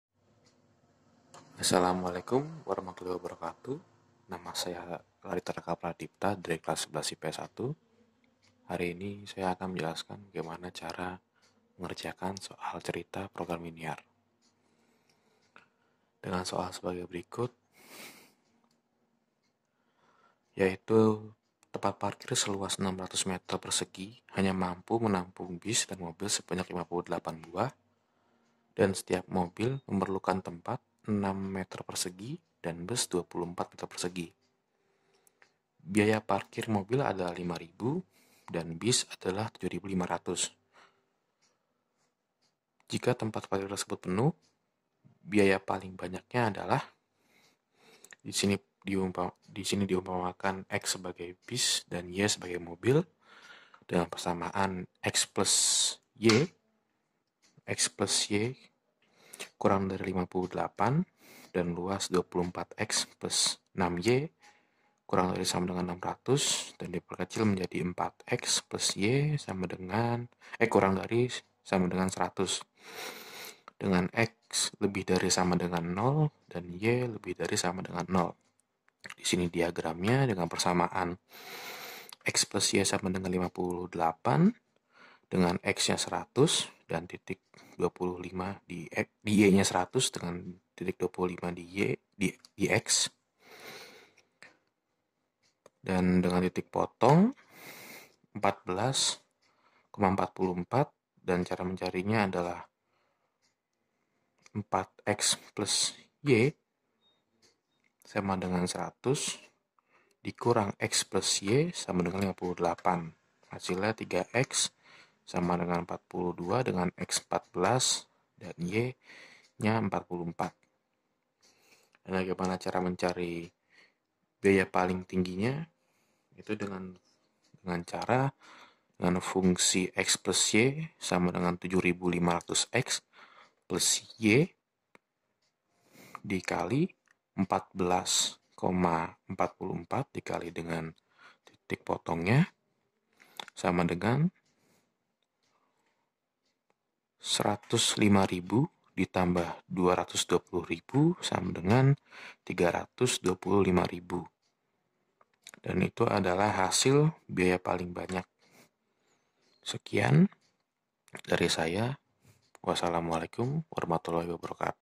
Cara MUDAH ngerjain Soal Cerita Program Linear | Radite XI IPS 1
Video dari Muh Rian Fadilah, S.Pd